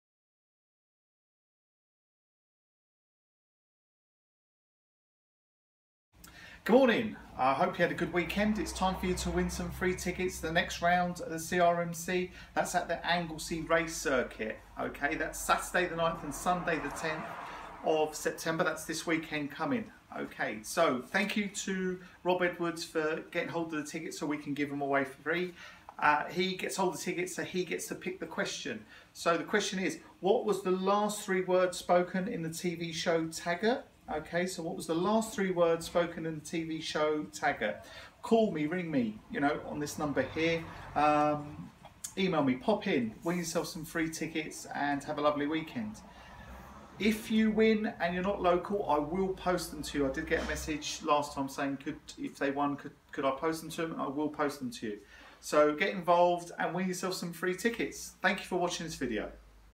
WIN TWO TICKETS TO THE NEXT ROUND OF THE CRMC 😎👍
Watch the video and answer the question to win 2 tickets to the next round of the CRMC sat 9th & sun 10 of September 😎👍